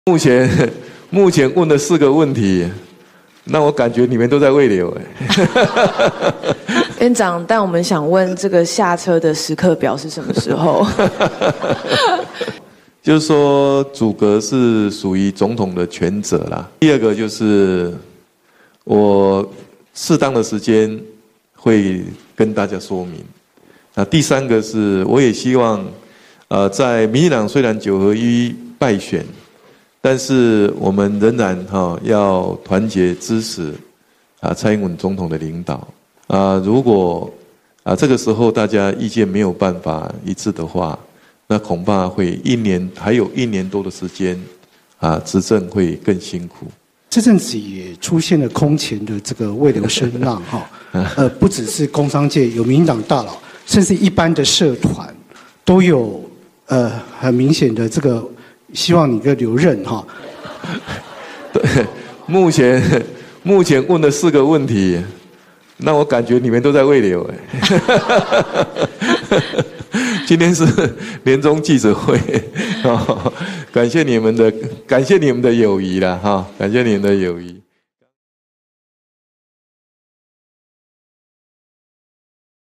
「預約請辭」有轉機？ 賴清德笑了：我感覺你們都在慰留
☛即新聞粉絲專頁
☛合作洽談信箱 （或私訊即新聞粉絲專頁）